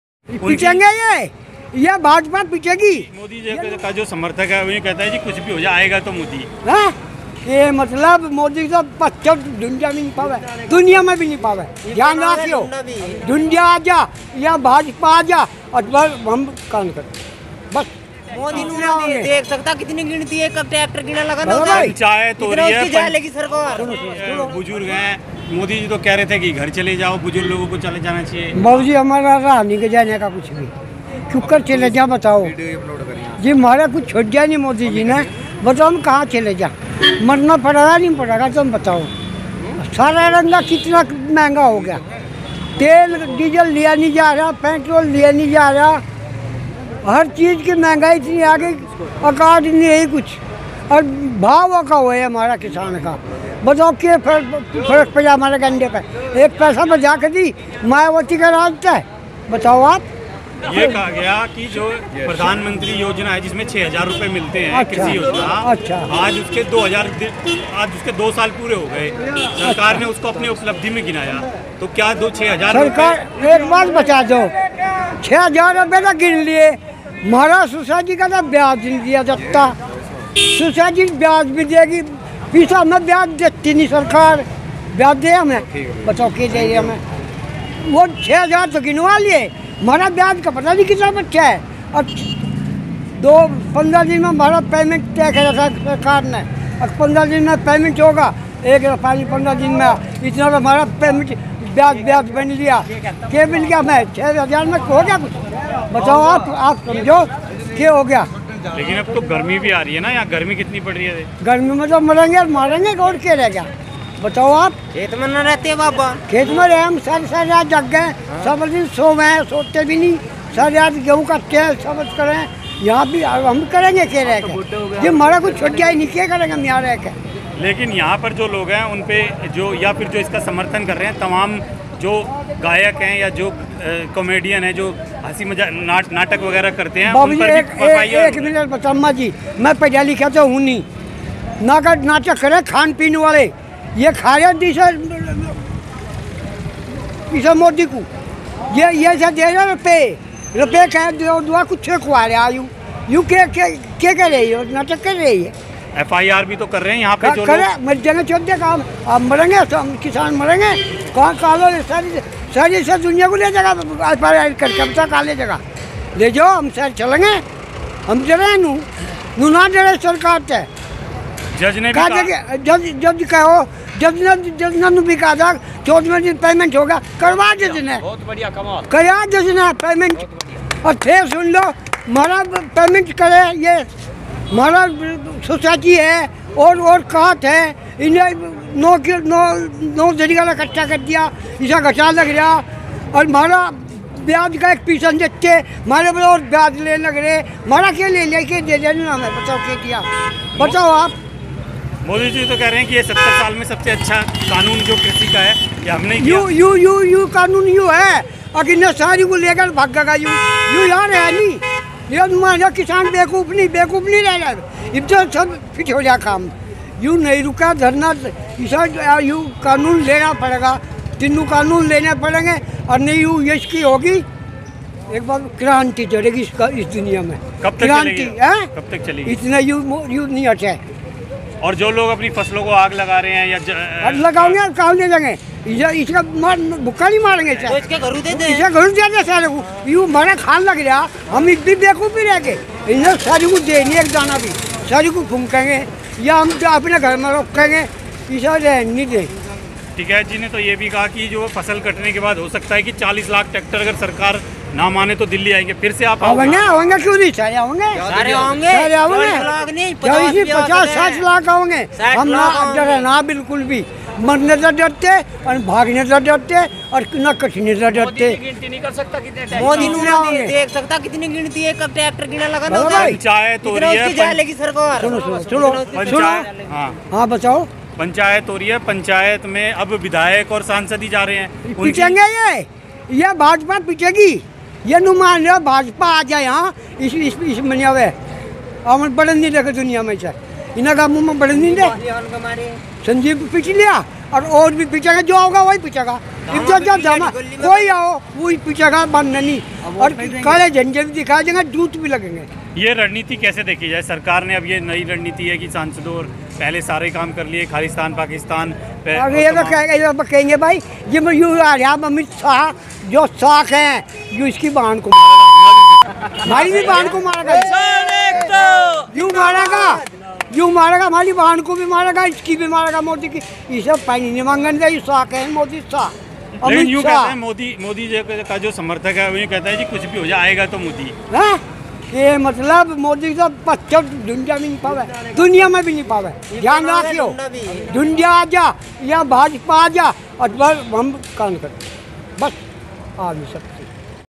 गाजीपुर बार्डर पर इस ताऊ ने मोदी और शाह को दिया करारा जवाब | Kisan Andolan Latest News.
गाजीपुर बार्डर पर इस ताऊ ने मोदी और शाह को दिया करारा जवाब | Gazipur border.

Your searches-
Kisan Andolan latest news,
farmers protest,
Kisan news,
Kisan Andolan new video,
Farmers breaking news